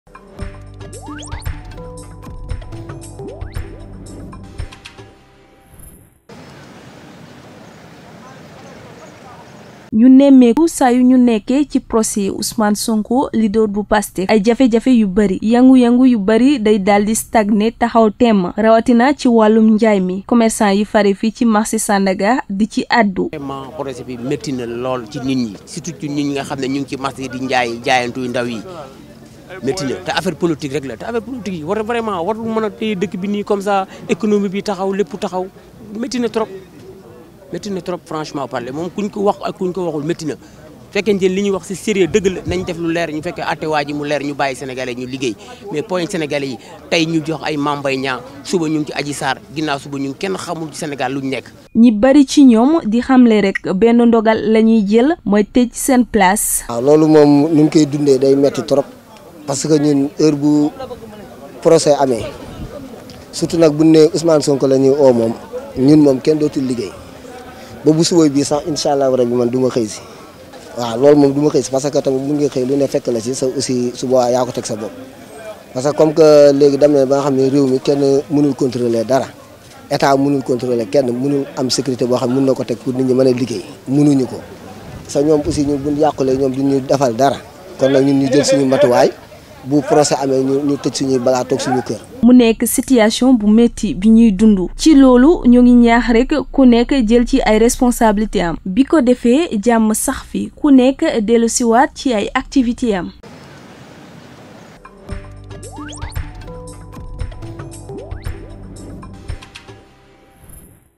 Conséquences du procès d'Ousmane Sonko: Sandaga obligé de fermer boutique
Les tensions causées par les procès d'Ousmane Sonko, leader de Pastef,  entraînent le plus souvent  de multiples conséquences, notamment pour les commerçants du marché de Sandaga, situé à proximité du tribunal de Dakar. Selon eux, la situation est extrêmement difficile à vivre. Afin de se protéger des risques, ils sont contraints de prendre des mesures qui ne sont pas non plus sans conséquence. Et, pour assurer leur sécurité, ils sont obligés de fermer leurs cantines.